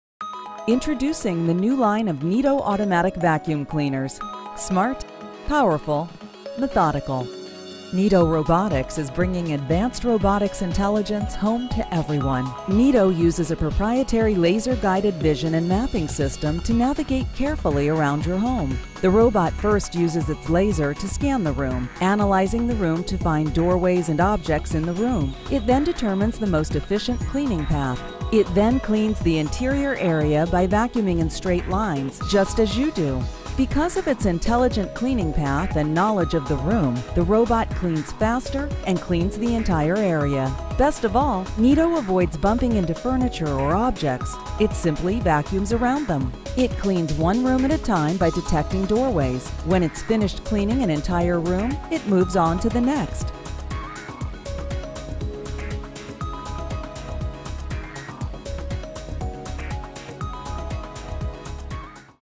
Automatic Vacuum Cleaner Features Laser Guide | Neato Robotics
Neato robot vacuums are smart.  Watch in this video as Neato sees objects in a room, and goes about cleaning the room in an intelligent manner, ensuring 100% floor coverage. Let this cleaning robot take over your vacuuming needs, allowing you more free time.

For more information about our automatic vacuum, please visit us

You can check out some of the latest Neato models

Transcript:

Introducing the new line of Neato automatic vacuum cleaners. Smart,
powerful, methodical. Neato Robotics is bringing advanced robotics
intelligence home to everyone. Neato uses a proprietary laser-guided vision
and mapping system to navigate carefully around your home.

The robot first uses its laser to scan the room, analyzing the room to find
doorways and objects in the room. It then determines the most efficient
cleaning path. It then cleans the interior area by vacuuming in straight
lines, just as you do.

Because of its intelligent cleaning path and knowledge of the room, the
robot cleans faster and cleans the entire area. Best of all, Neato avoids
bumping into furniture or objects. It simply vacuums around them.

It cleans one room at a time by detecting doorways. When it's finished
cleaning an entire room, it moves on to the next.